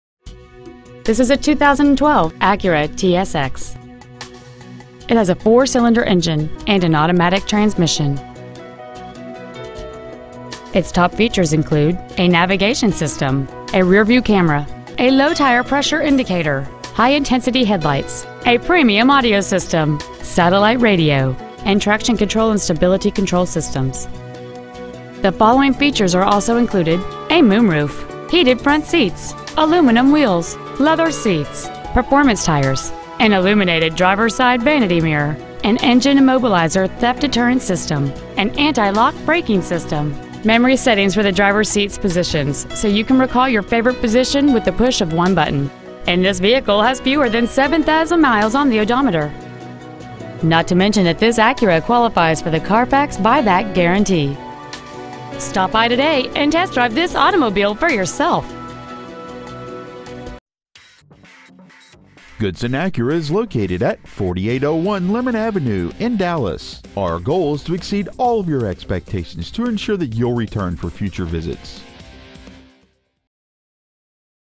2012 Acura TSX Irving TX
Year : 2012
 Make : Acura
 Model : TSX
 Engine : 4 Cyl - 2.40
 Trans . : Automatic
 Exterior : Crystal Black Pearl
 Miles : 6,237
 Interior : Ebony
 Stock : CC017864

 2012 Acura TSX Dallas TX